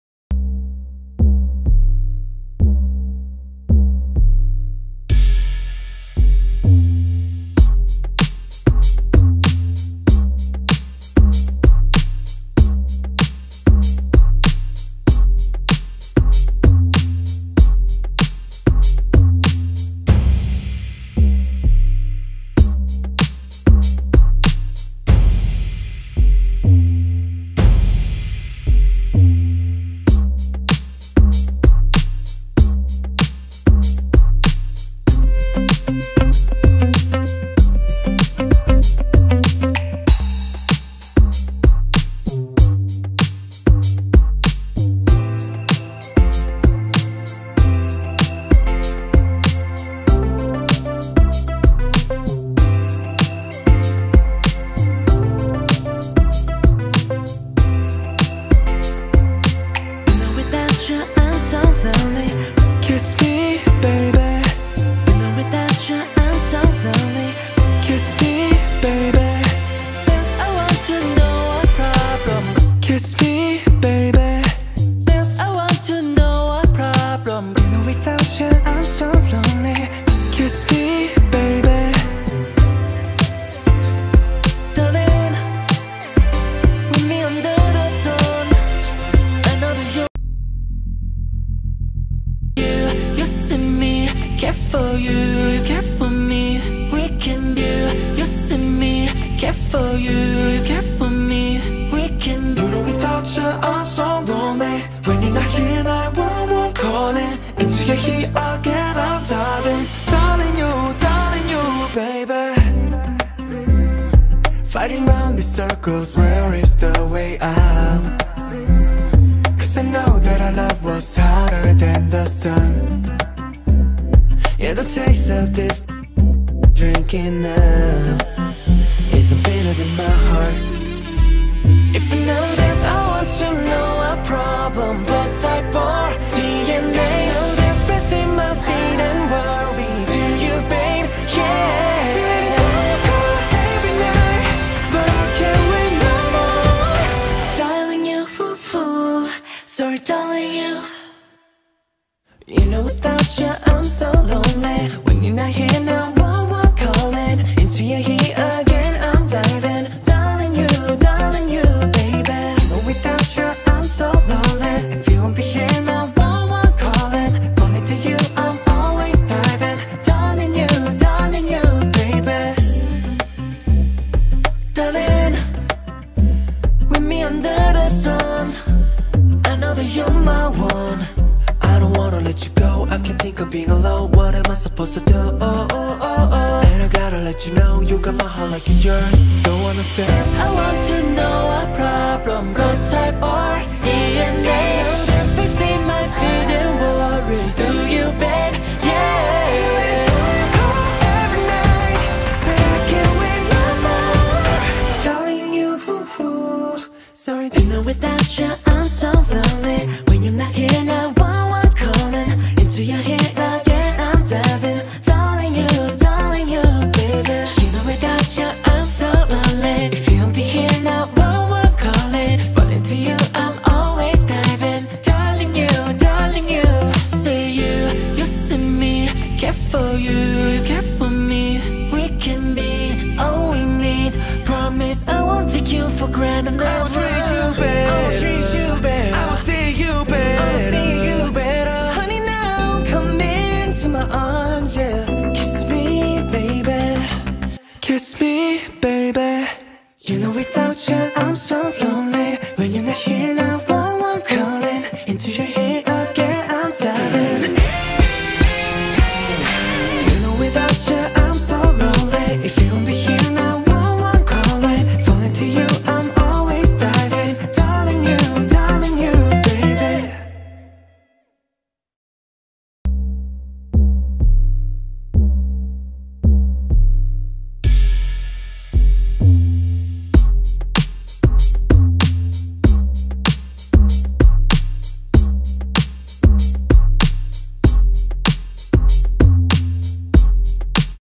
Box Hill Sydney NSW Australia Suburb (2765)
Boxhill - Box Hill newly developed suburban area in Sydney New South Whale. This newly developed area is located approximately 40 kilometres northwest of Sydney CBD

Australia Travel Guide
Australia Travel
Australia Top
Australia best place to live
Travel Video
Australia Sydney